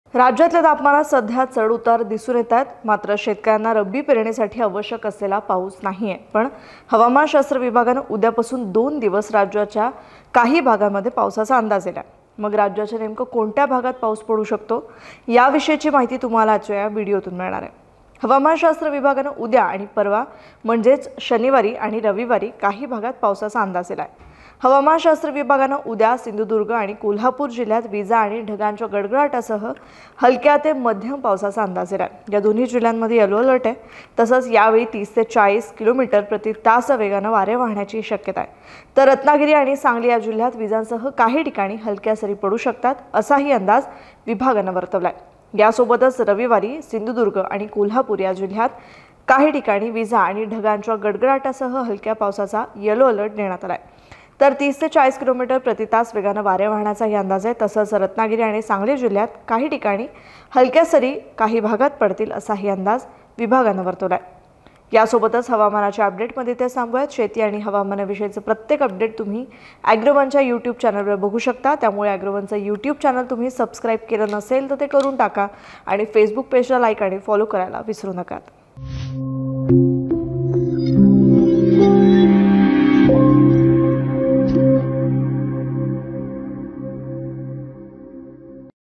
Maharashtra Rain: तापमानातील चढ उतार सुरुच | Agrowon | ॲग्रोवन
राज्यातील तापमानात सध्या चढ उतार दिसून येत आहेत. मात्र शेतकऱ्यांना रब्बी पेरणीसाठी आवश्यक असलेला पाऊस नाही. पण हवामान विभागाने उद्यापासून दोन दिवस राज्याच्या भागात पावसाचा अंदाज दिला. मग राज्याच्या नेमकं कोणत्या भागात पाऊस पडू शकतो? याविषयीची माहिती तुम्हाला या व्हिडिओतून मिळेल.